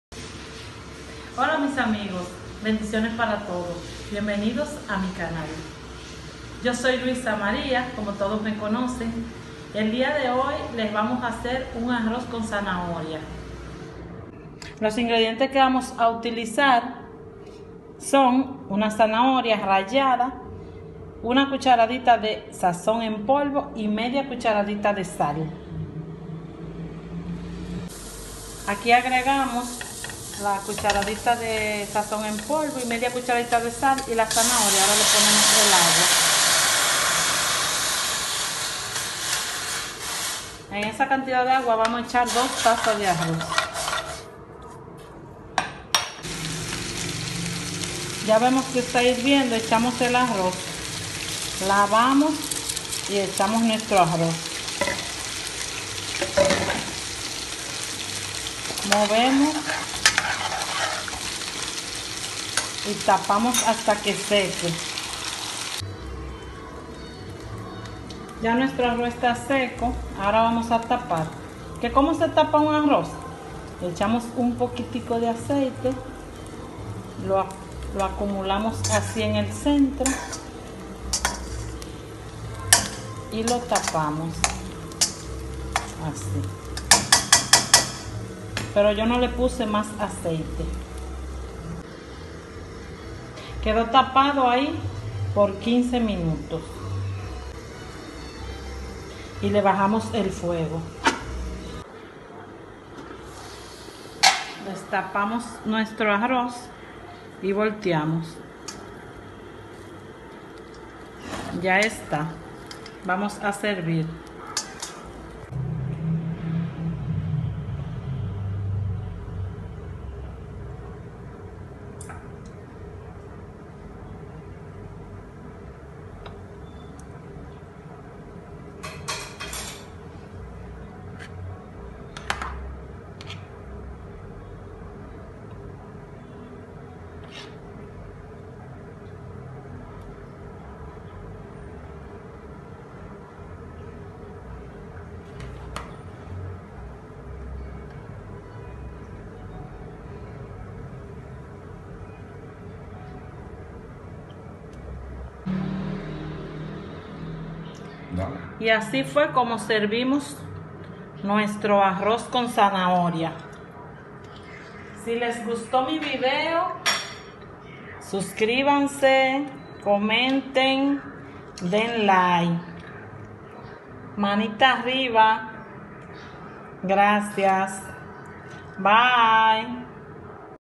Cómo hacer arroz con zanahoria, fácil y rápido.
Arroz con zanahoria, bien fácil de hacer,es muy sabroso y saludable,pues. Los invito a que lo prueben hacerlo porque ese arroz con zanahoria quedó de olor.

    Ingredientes .

1 zanahoria
2 tazas de arroz.
Sal a su gusto.yo le puse media chat.y me quedó perfecto.
1/2 chat. De sazón en polvo .